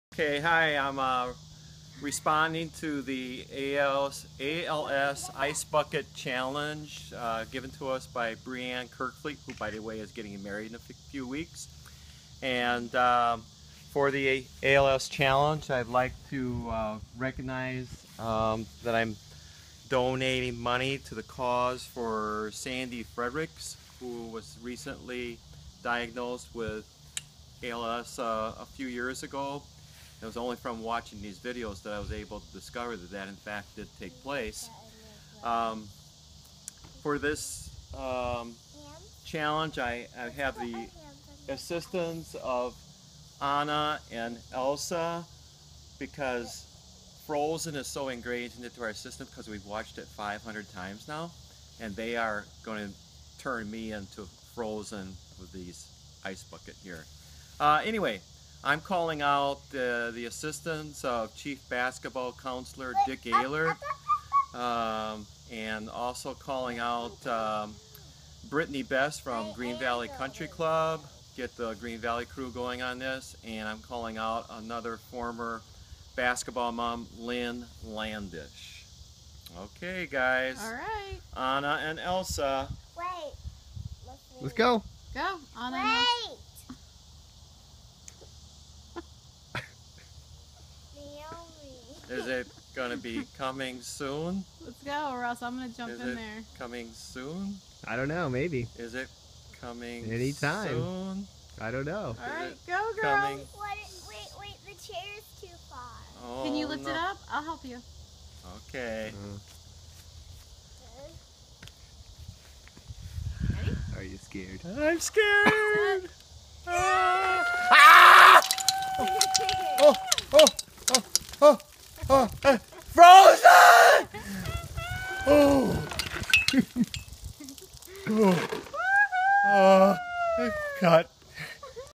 ALS IceBucket
This video is in response to the ALS Ice Bucket Challenge from Breanne Kerkvliet. I will be donating in honor of Sandy Fredericks. I learned from watching these ALS Ice Bucket Challenge videos that Sandy was diagnosed with ALS a few years ago. Sandy and her husband Tom did many hours of volunteering there time in fund raising efforts for Park High Girls BB teams in the 1990's. Their daughters Danielle, Nicole, and Missy all played for me in the 1990's. While I was the Activities Director, I worked with their other daughter Michelle who is currently teaching at Park. For the challenge, I am calling out Chief Basketball Counselor Dick Ehlert, and a former basketball mom, Lynn Landish. I am also calling out Brittany Arnost Best and my other co-workers at Green Valley Country Club. I was assisted in this challenge by Princess Anna and Princess Elsa - Ava and Naomi - Angela Eller, Bob Eller, Stephanie Kerkvliet